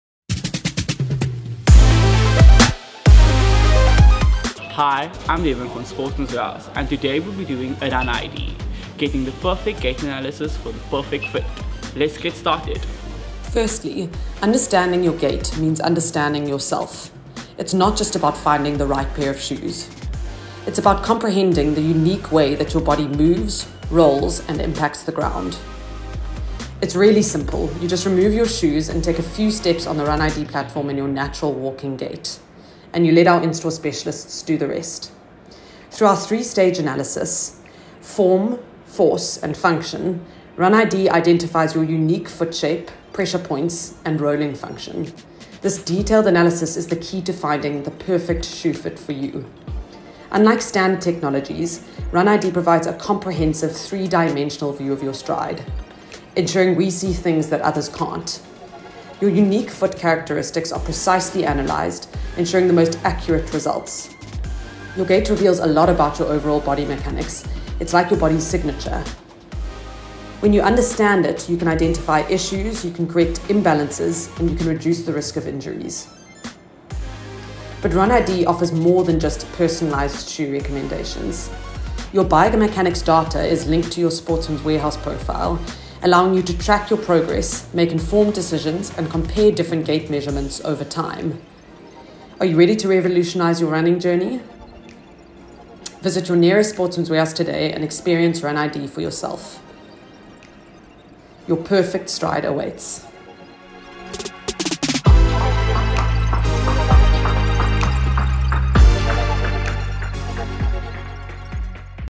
RUN-id Advanced Gait Analysis | Sportsmans Warehouse Specialty Services
We determine the best shoe fit for your foot by doing a biomechanical analysis of your gait, foot pressure, foot rolling behaviour, and arch type.

Exclusive to selected Sportsmans Warehouse stores.

We are not giving medical advice. This should always be sought from a medical professional. Instead, we provide this service to find the best shoe fit for your foot.